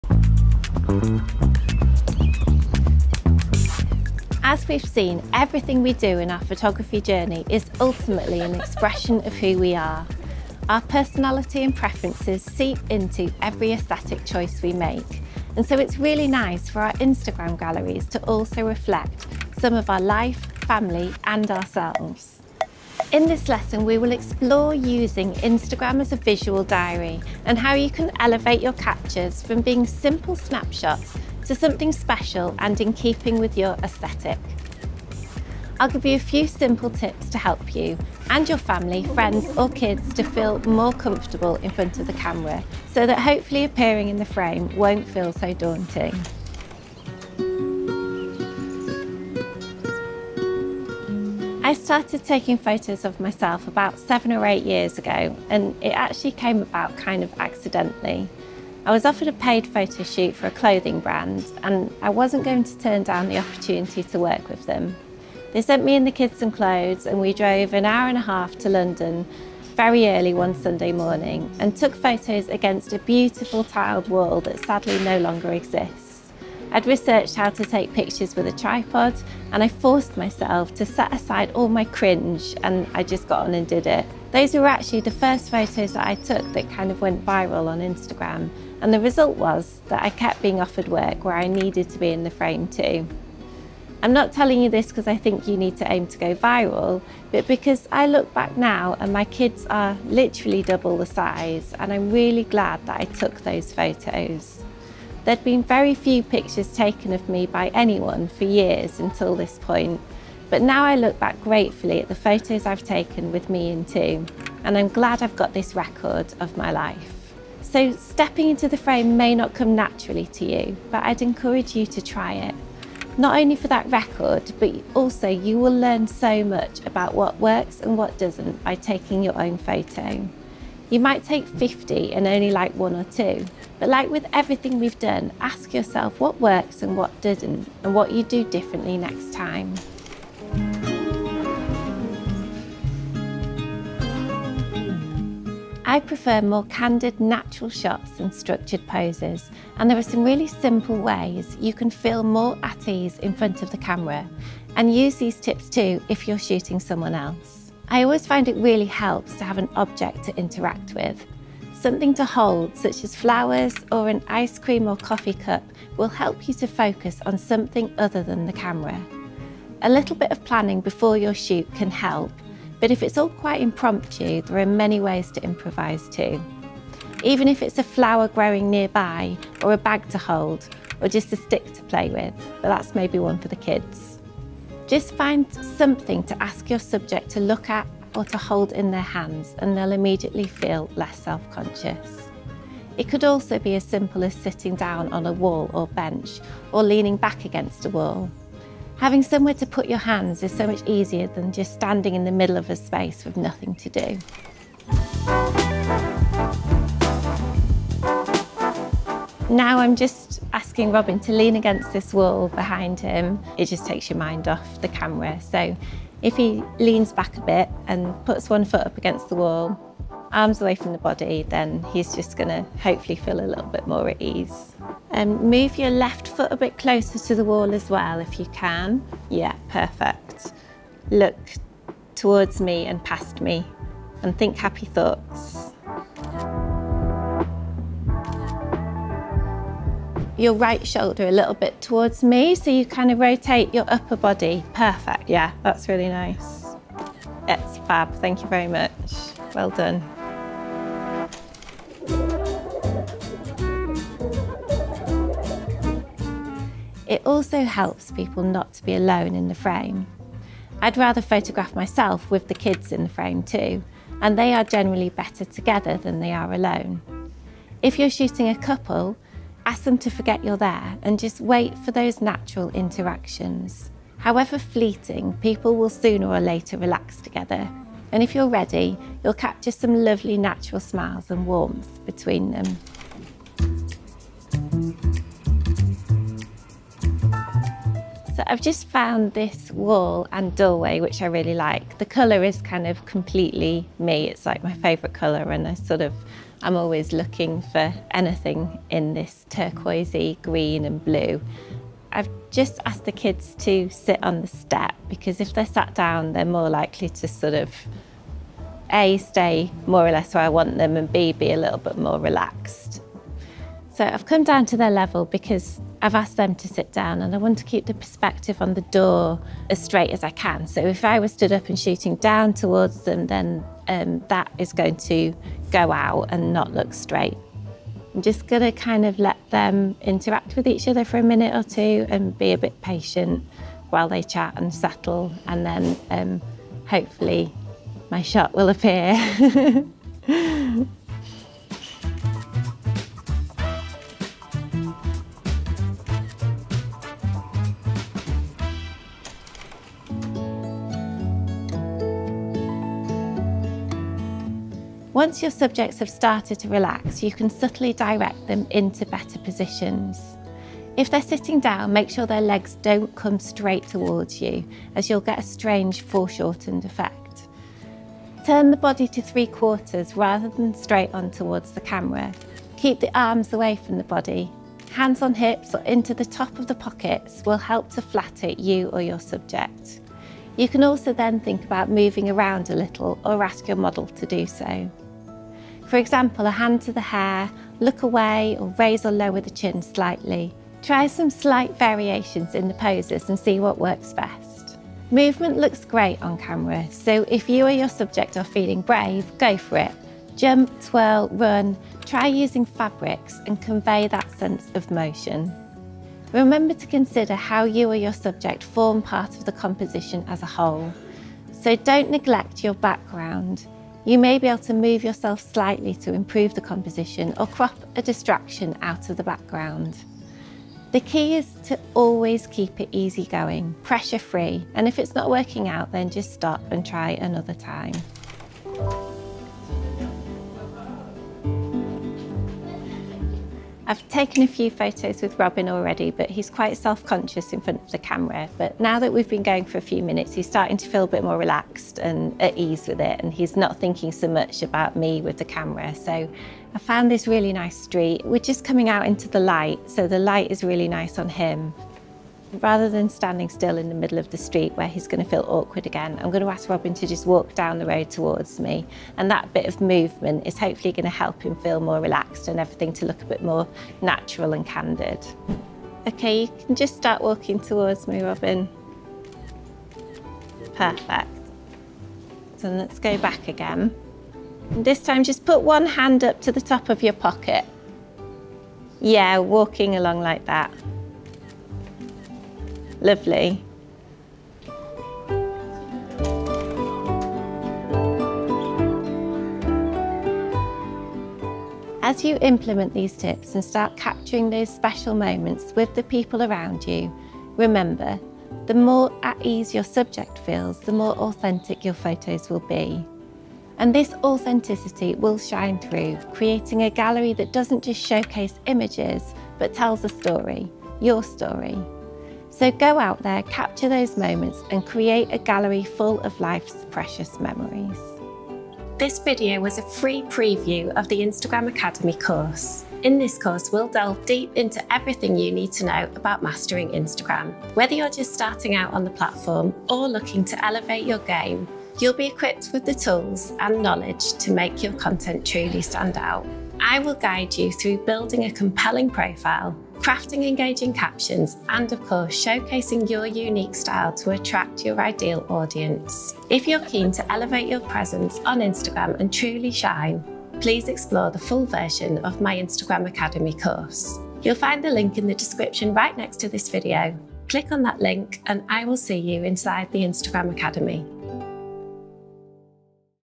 How to Direct and Pose for Photos
Discover the proven formula to create an irresistible Instagram account

In this video, you’ll discover the secrets to making subjects feel comfortable and authentic in front of the lens.

🔎 What you’ll learn:
- Creating a Visual Diary on Instagram: Learn how to use Instagram as a visual diary that captures and reflects your life.
- Master Posing & Directing: We'll show you how to guide subjects or yourself to look comfortable in front of the lens.
- Candid Photography: Uncover the secret to capturing spontaneous moments with different techniques.
- Using Props & Scenery: Find out how the simplest objects and backgrounds can transform your photos into engaging stories.
- Composition & Perspective: Elevate your photography with expert tips on framing and perspective.

⌚CHAPTERS:
0:00 Introduction
0:47 Taking Photos of Yourself
2:21 How to Feel More Comfortable
4:12 Taking Photos of Multiple Subjects
5:49 Directing Models
8:06 Conclusion